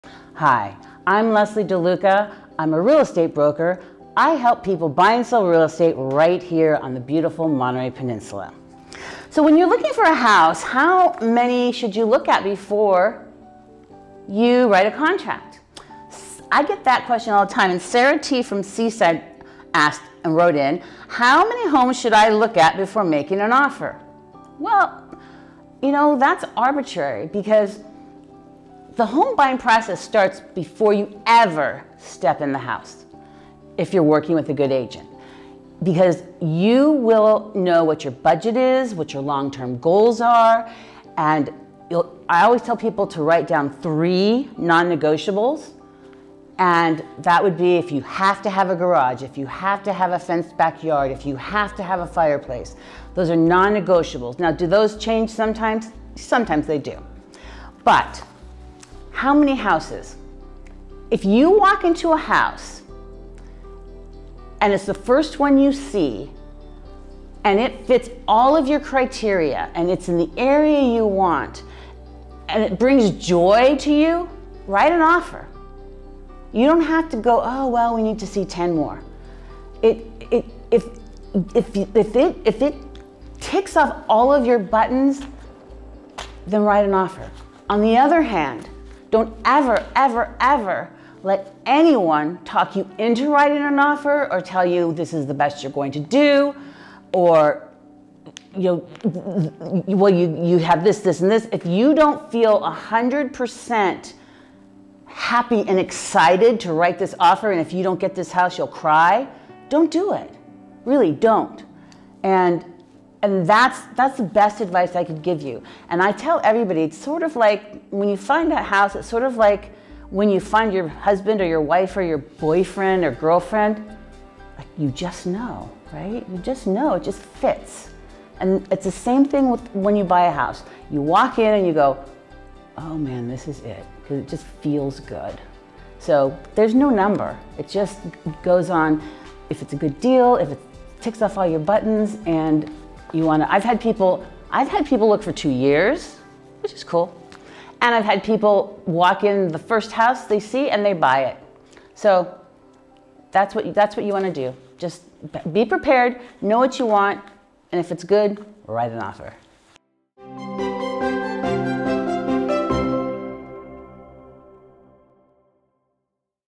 How Many Houses Should I look At Before Writing An Offer?
How many homes to look at before you write an offer? What do you think? Leslie DeLuca answers questions from the community on the subject.

Leslie DeLuca | Broker Associate
Compass Carmel
CaDre01364722